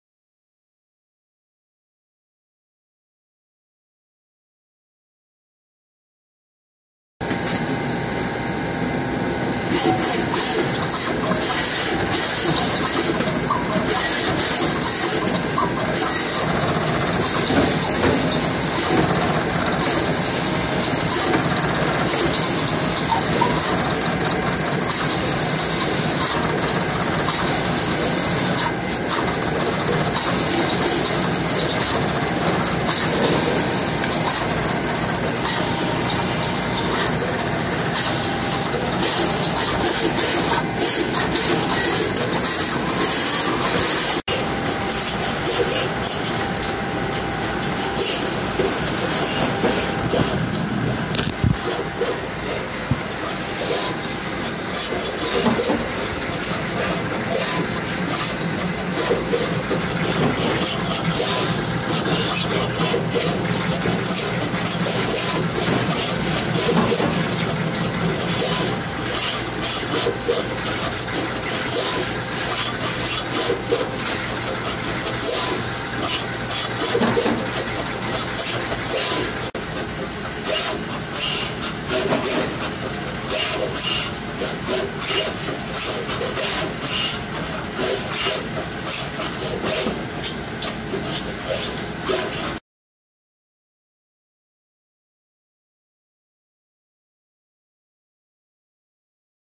ACME Fuji SMT assembly line
Fuji SMT assembly line help customer get their quality boards back from ACME PCB Assembly.